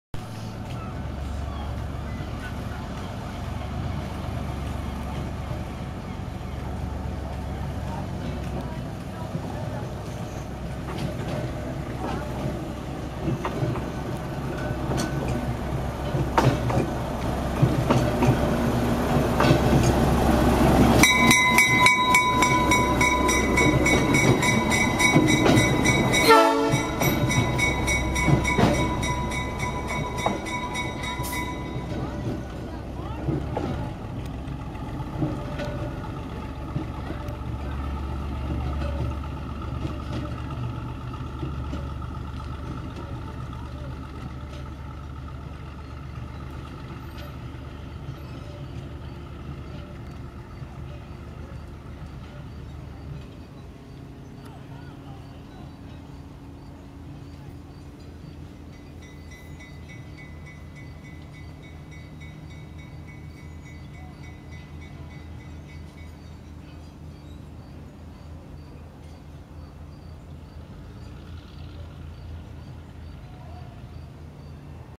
Maine Narrow Gauge Railroad diesel
A diesel locomotive on the 2ft gauge Maine Narrow Gauge railroad in Portland, Maine.